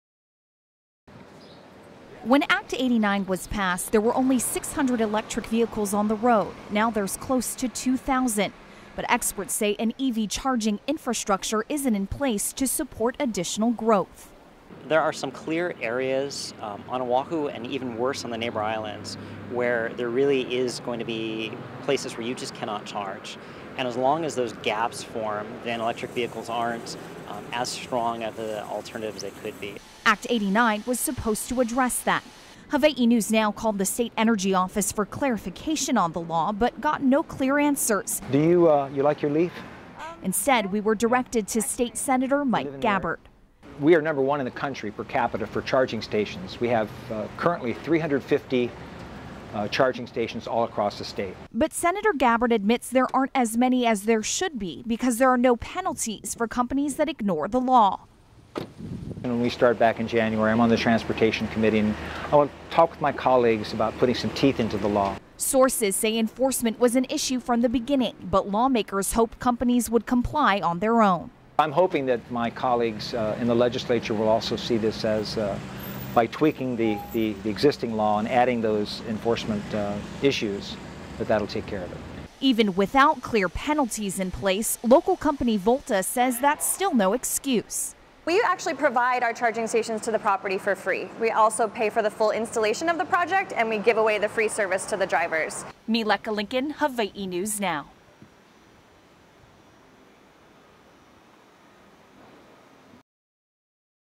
Electric Vehicle Charging Stations
The popularity of electric vehicles are growing, but not the infrastructure for charging stations.